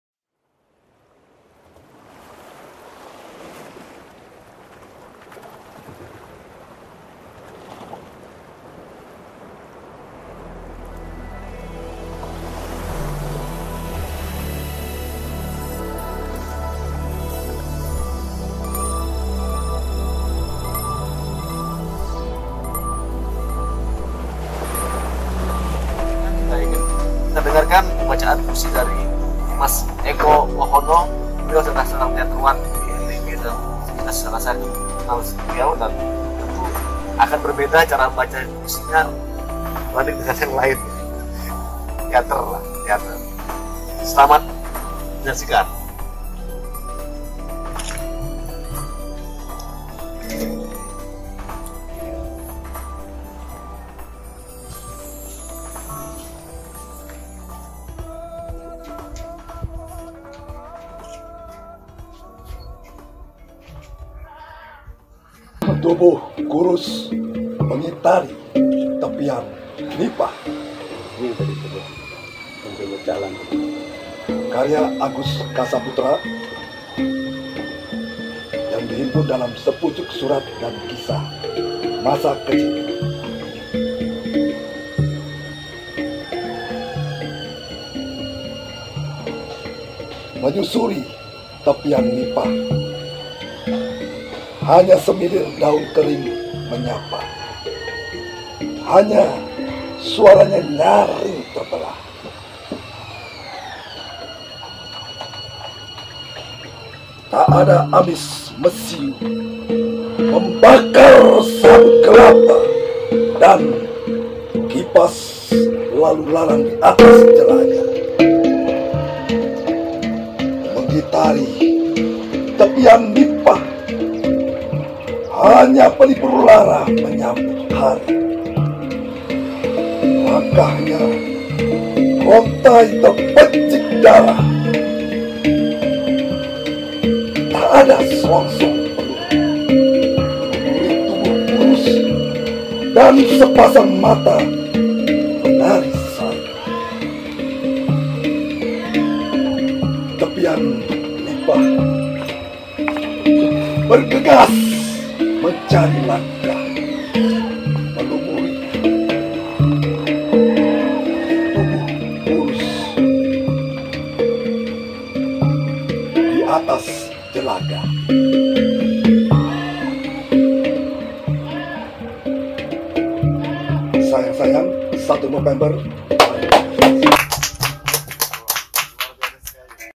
Pembacaan Puisi Tubuh Kurus Mengitari Tepian Nipah karya Agus K Saputra oleh R. Eko Wahono
Puisi di atas terhimpun dalam buku antologi puisi Sepucuk Surat Dan Masa Kecil, terbitan pertama 2020.